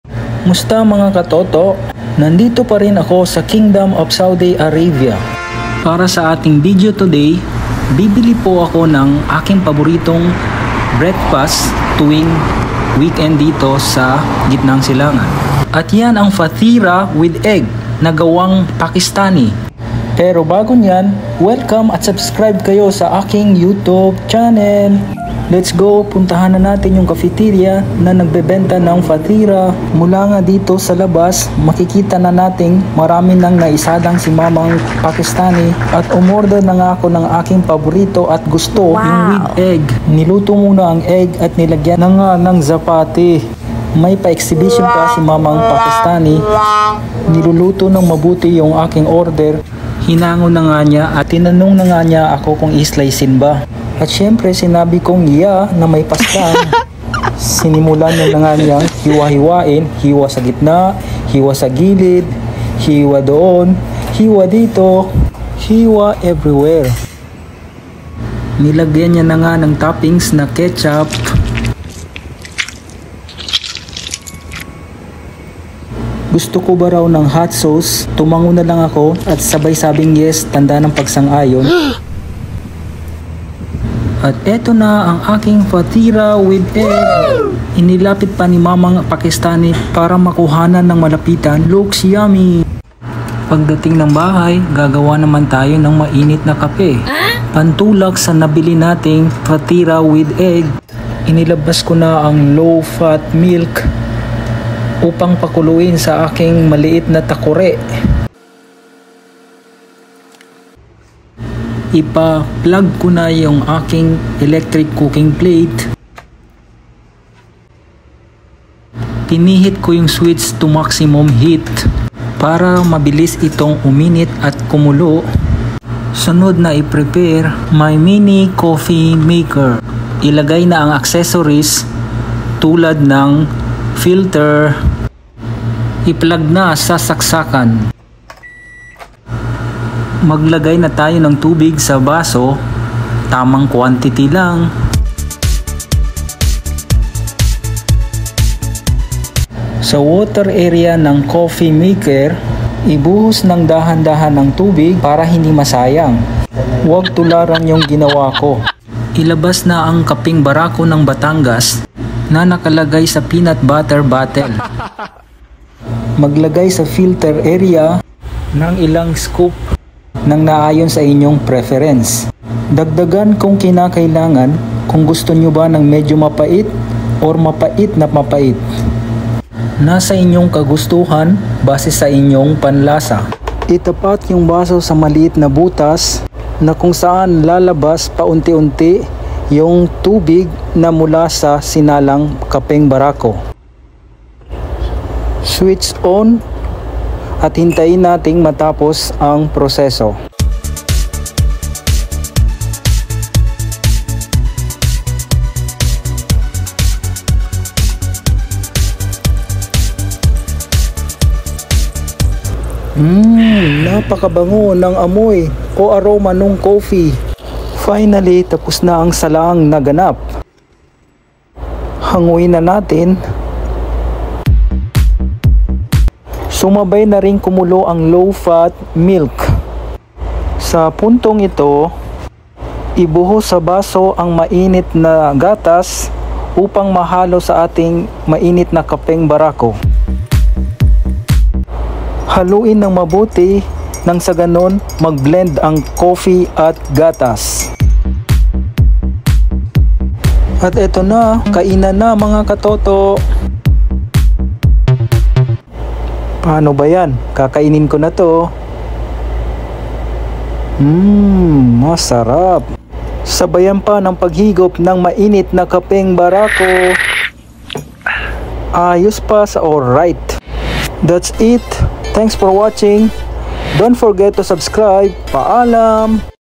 Fatherrah w/egg, gawang Pakistani and Kapeng Barako w/low fat milk.
Life in KSA, isa sa mga pinagkakaabalahan tuwing weekend or hijasa sa wikang Arabic.